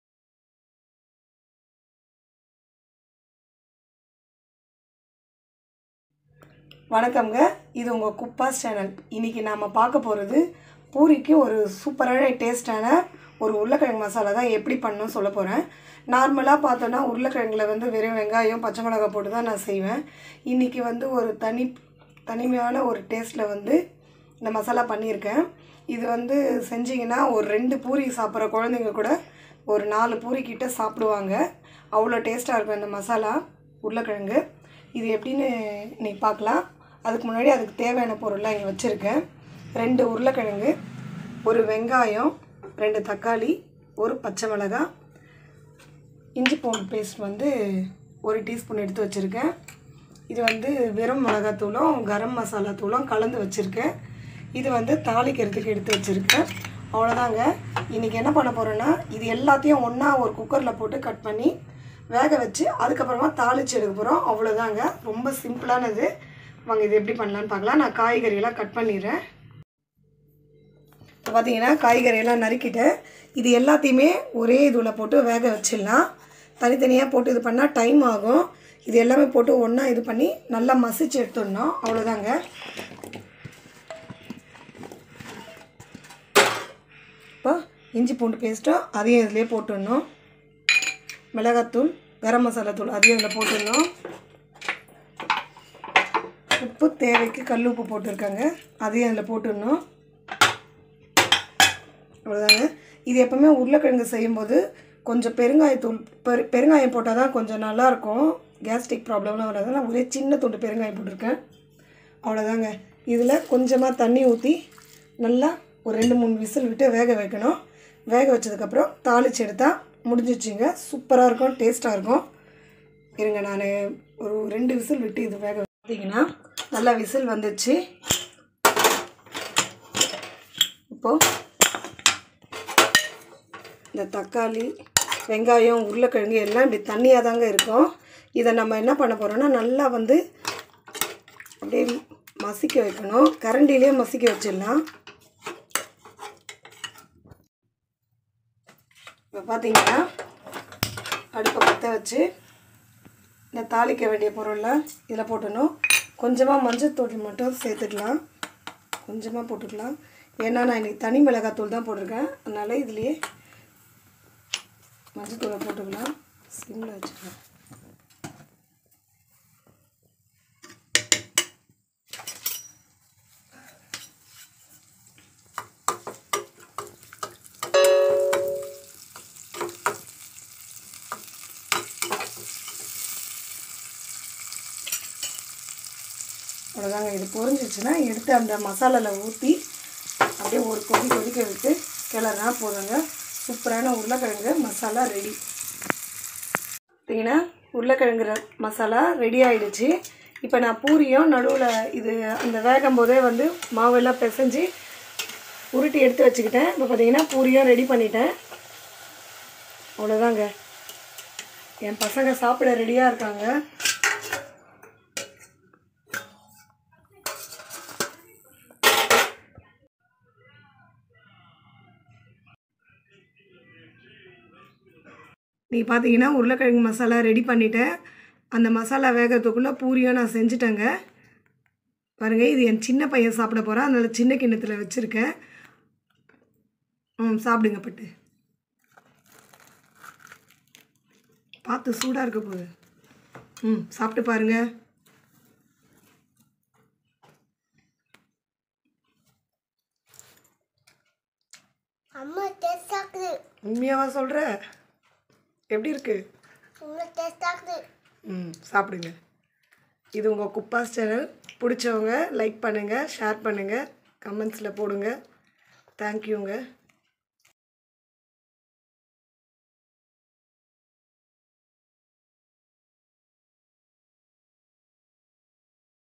பூரிக்கு ஏற்ற உருளைகிழங்கு மசாலா..2 பூரி சாப்பிடுறவங்க 4 பூரி சாப்பிடுவாங்க...
urulaikilangu,thakkali 2,vengayam 1,pachai milagai 1